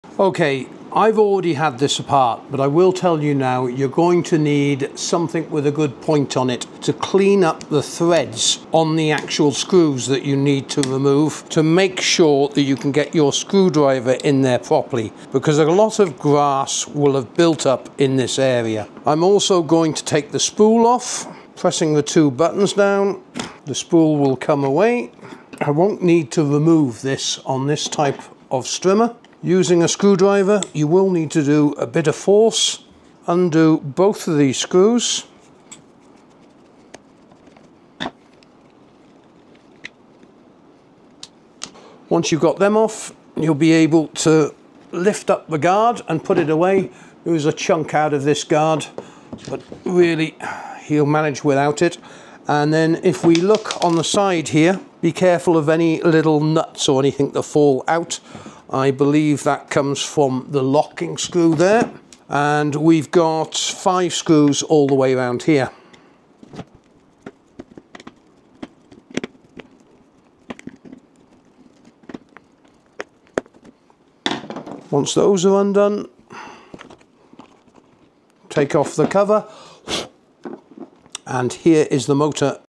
How to Dismantle a Cordless Strimmer or Grass Trimmer? Removing the Cover and Guard
In the full video, I show you how to diagnose and repair a common fault on a cordless strimmer (grass trimmer or garden strimmer) by replacing the brushless motor. Whether you are diagnosing a motor fault or need to replace internal components, getting the housing open without losing parts is the first step. If you are struggling to get your garden appliances ready for the season, the full video will help you get inside the machine to find the fault.

I also expose the massive battery scam that cordless tool manufacturers like Makita, Bosch, DeWalt and others are pulling. They trap you in their brand with incompatible 18V battery systems, forcing you to buy new tools instead of repairing old ones. It's built-in obsolescence, and it's an environmental disaster.